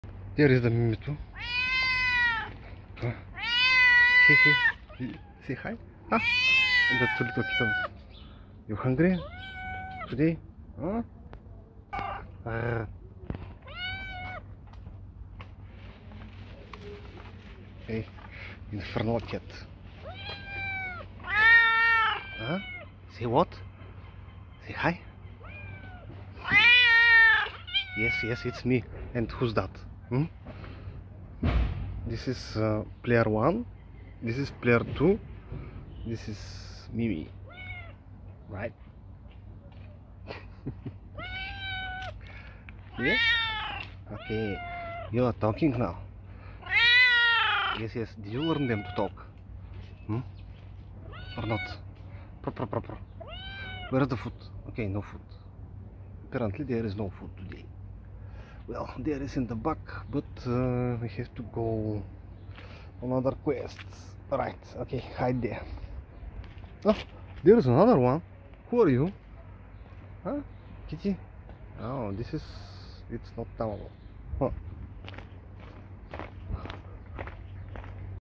Mimi and Her Offspring - Cats
Neighbour cats. From calm streets to majestic villages, follow along the thrilling adventures, encountering cute, fun and playful cats. Digital cat safari.